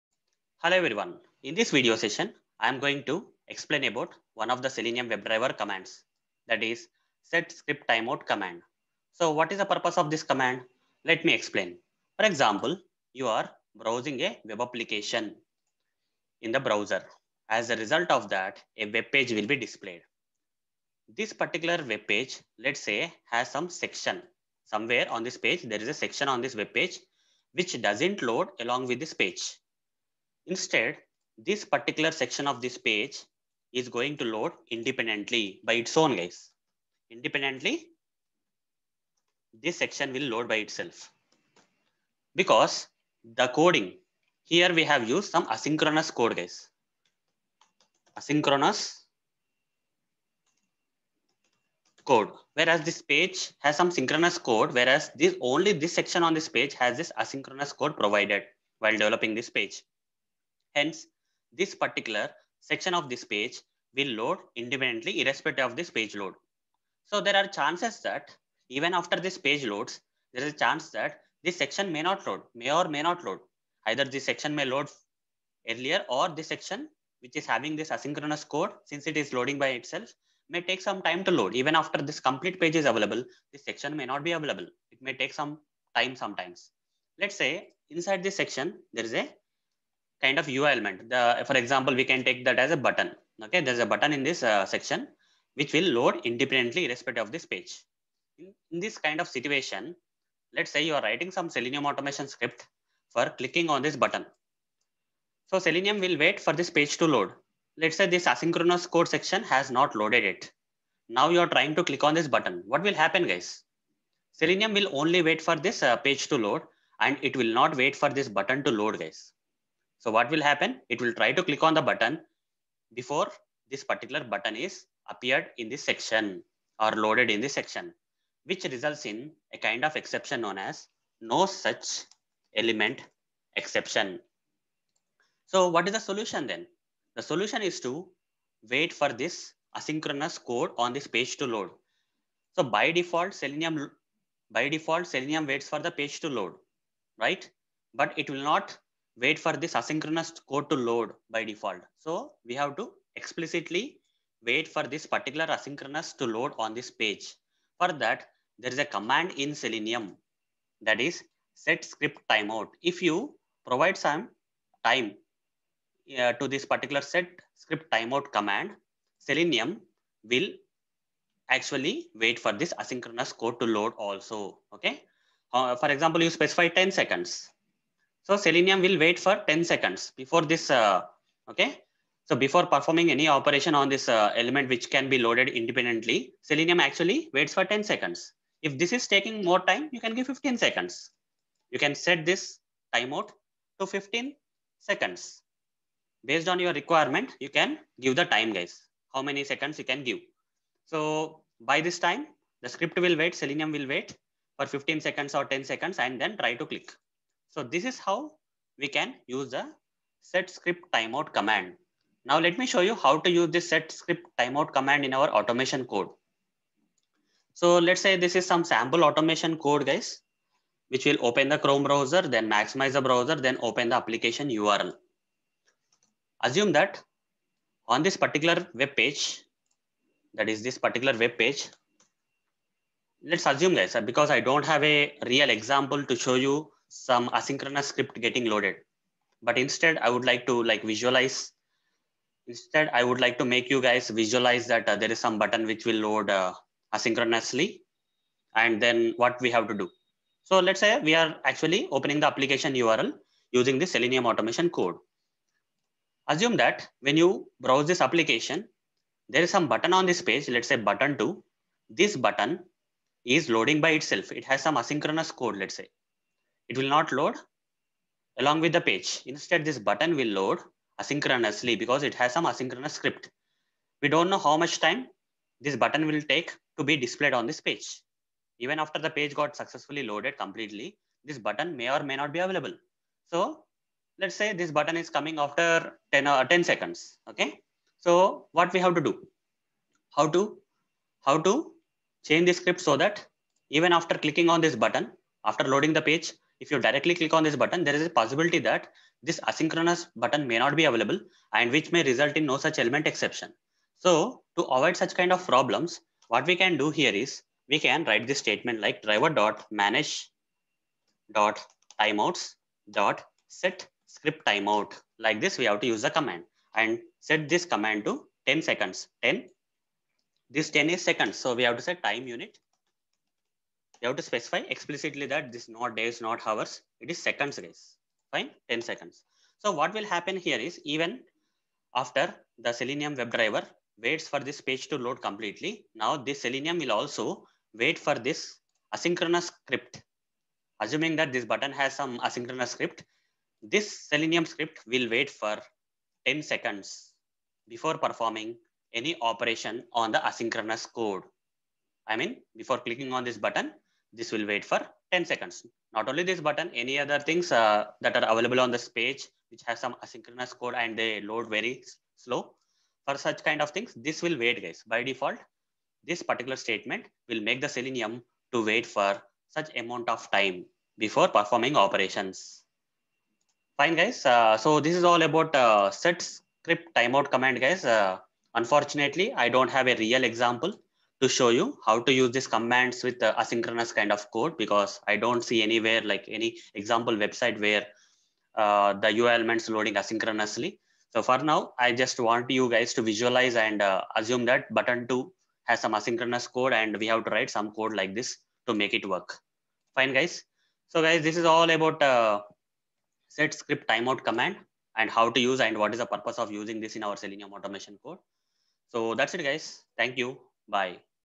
setScriptTimeout() Command - Selenium WebDriver
In this video, I have practically demonstrated one of the Selenium WebDriver commands i.e. setScriptTimeout() Command